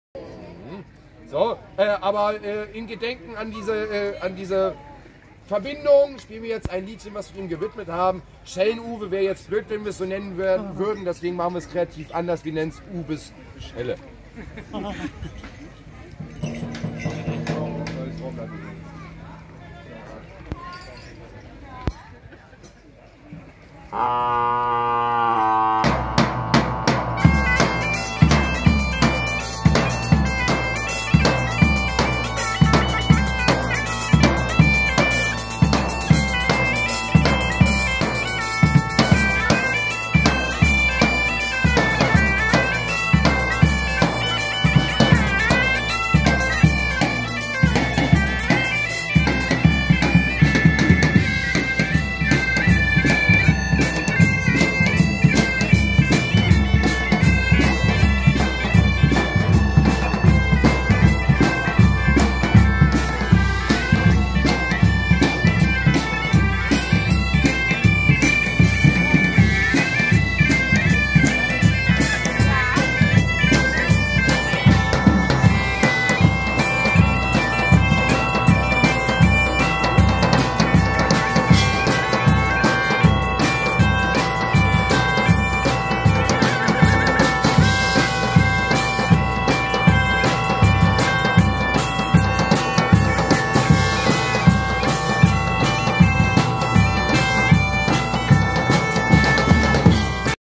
Die Mittelalter Band Piro Zores aus Erfurt beim Mittelaltermarkt im Schlosspark in Gersfeld